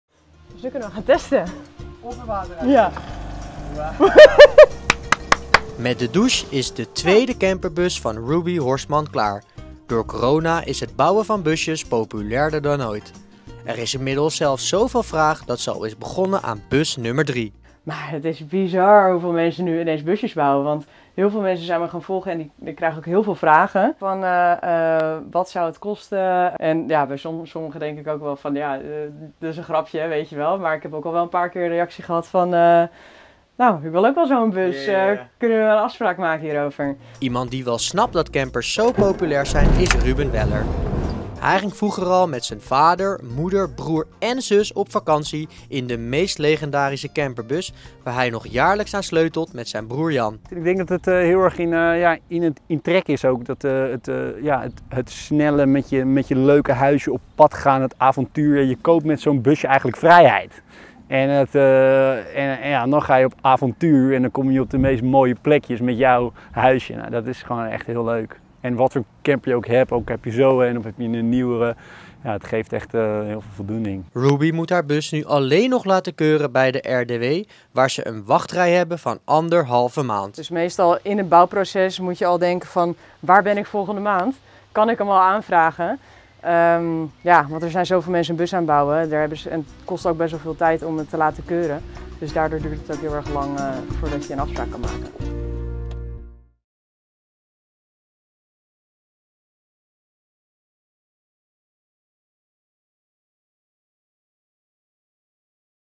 Ongekend populair: busjes ombouwen tot camper
Een nieuwe corona-rage: een bestelbus ombouwen tot camper. Ruby kan haar geluk niet op: 'Was mijn droom'. Ze wordt overspoelt met mensen die ook zo'n bus willen. Waar ze dit eerst als hobby in de vrije uurtjes deed, is ze inmiddels al begonnen aan haar derde project. 'Het is echt bizar hoeveel mensen mij om informatie vragen.'